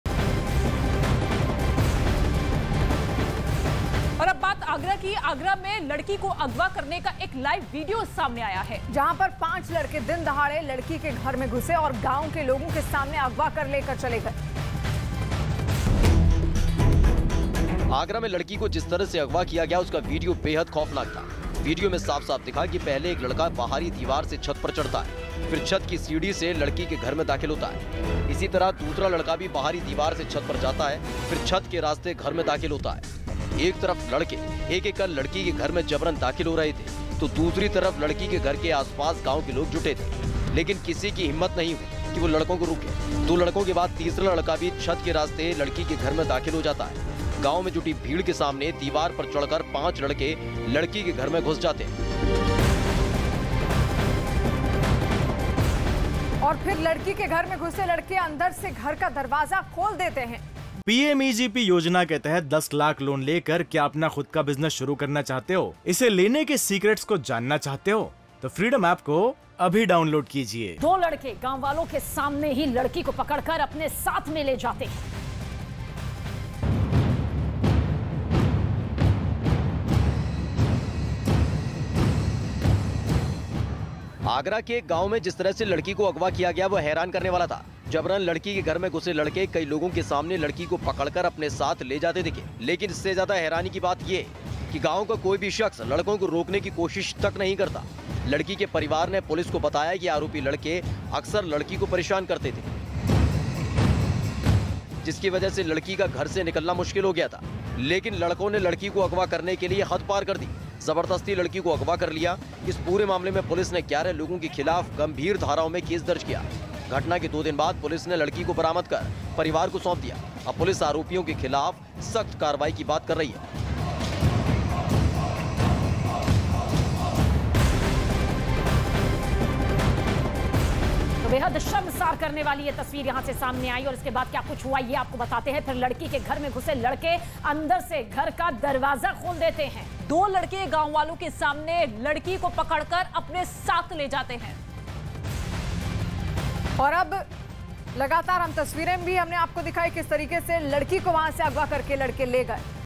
Breaking News Agra में लड़की का अपहरण, दिन में घर में घुसकर किया अगवा
Breaking News: Agra में लड़की का अपहरण, दिन में घर में घुसकर किया अगवा | Agra News | UP News

Breaking News: आगरा में एक युवक दिनदहाड़े घर में घुसकर किशोरी को अगवा कर ले गया। घटना के वक्त भीड़ जुट गई, लेकिन कोई बचाने की हिम्मत नहीं जुटा सका। बाद में पुलिस ने किशोरी को बरामद कर लिया है।